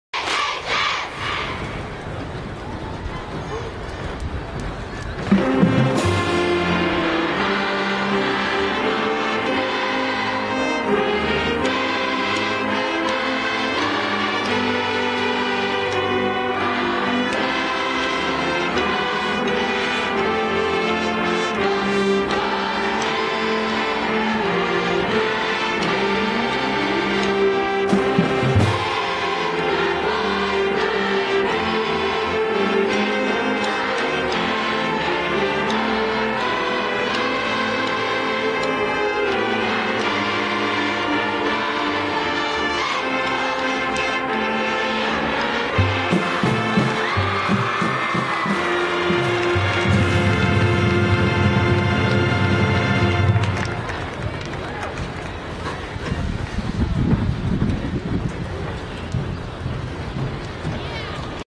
The John Jay Mustang Band performs at the Taft football game on Thursday, October 11, 2012.